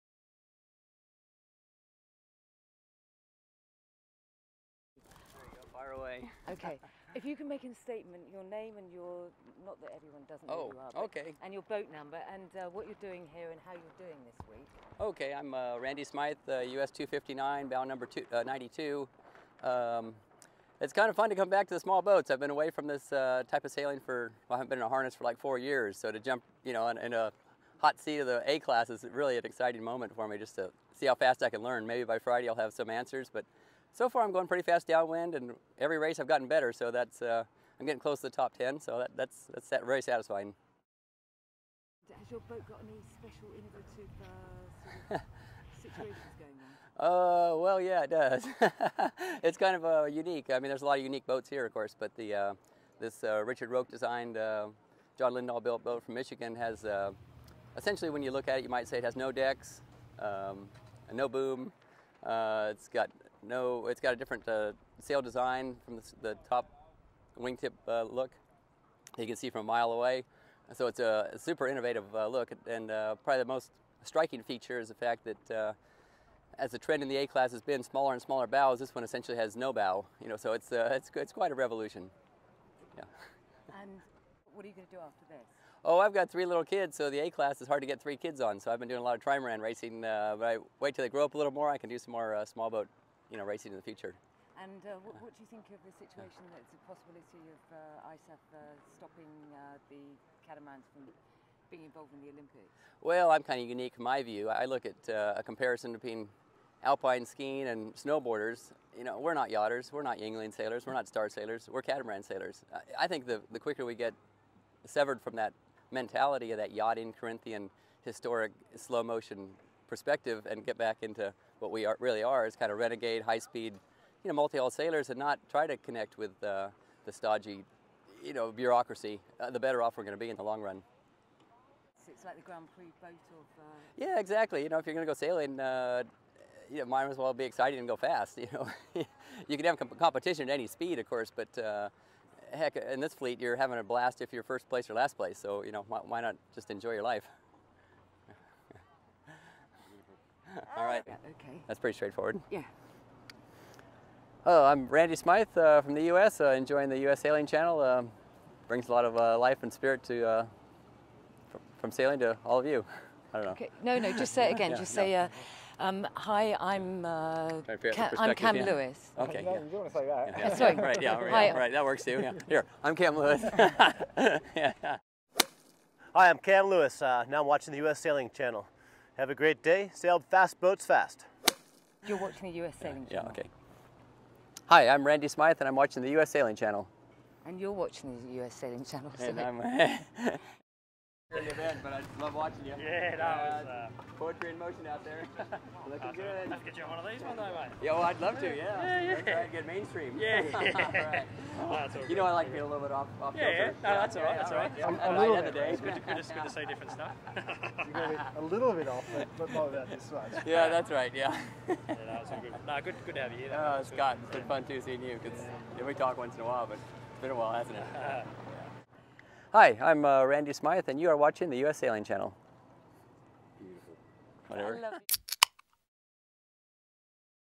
Randy Smyth
Randy Smyth talks about A-Class cats ISAF and cats in general.